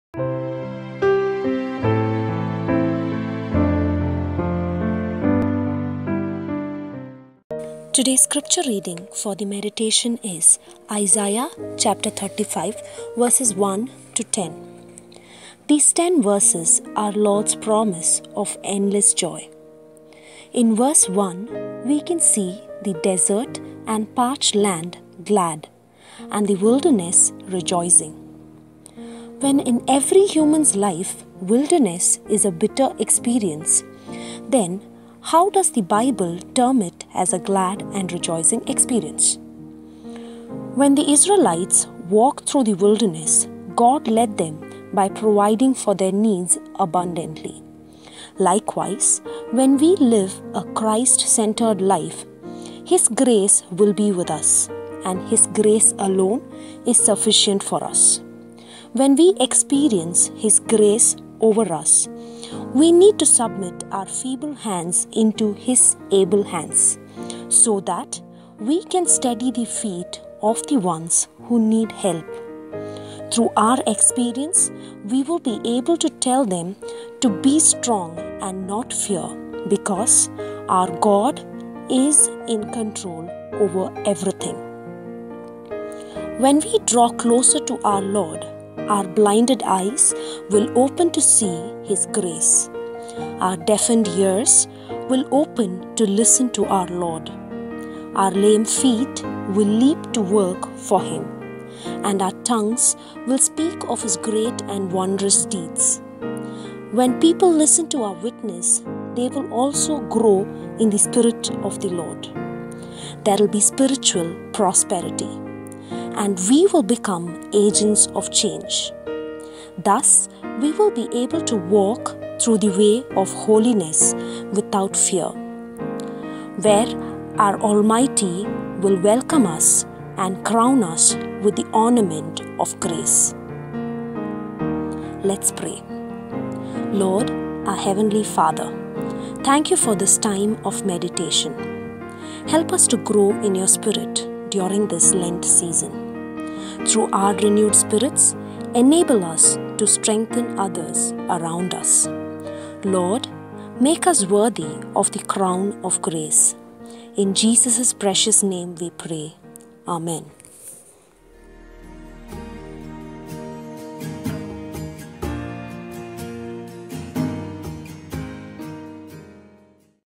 Day 14 Lenten Evening Meditation
Mar Thoma Yuvajana Sakhyam - Vanitha Forum presents
Scripture Passage: Isaiah 35: 1 - 10
Message by Jissa A. Varghese: Bethel MTC, K.R. Puram
Please share and spread the word..